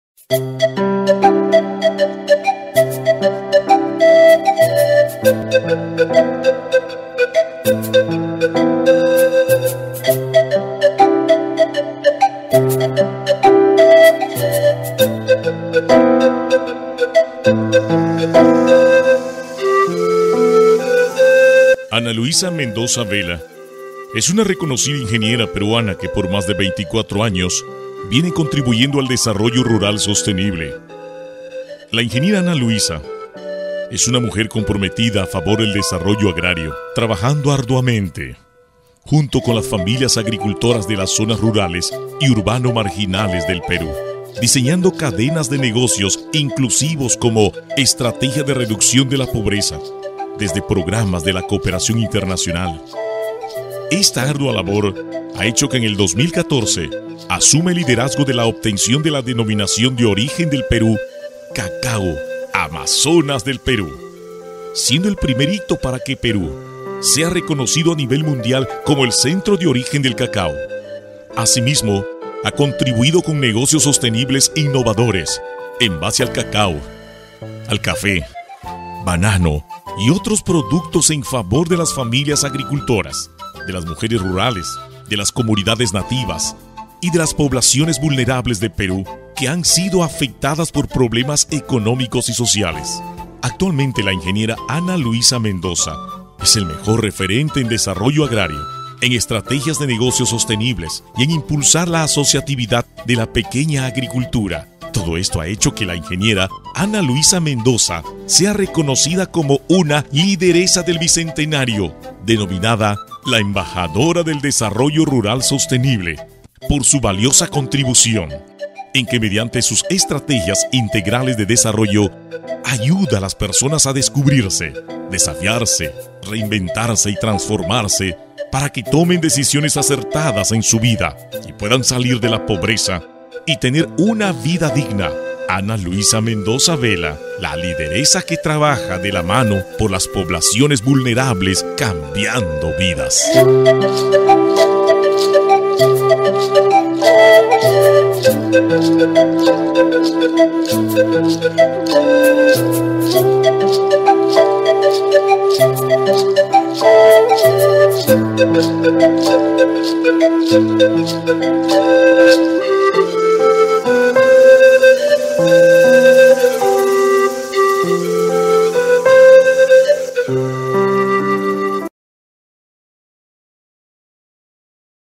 ANA LUISA TRABAJA CAMBIANDO VIDAS: Lideresa comprometida con las poblaciones vulnerables!
Ana Luisa Mendoza es una reconocida ingeniera peruana que por 24 años aporta al desarrollo rural sostenible trabajando desde programas de la cooperación internacional con vocación, compromiso y pasión, por lo que ha sido RECONOCIDA como LIDERESA DEL BICENTENARIO por ser una persona influyente en el desarrollo rural sostenible.